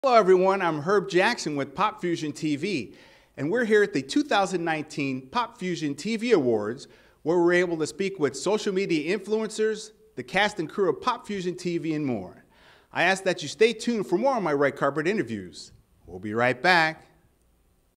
Demo reel
Demo reel recorded at the 1817 Film Studios in Burbank, CA. Recorded for and by @popfuziontv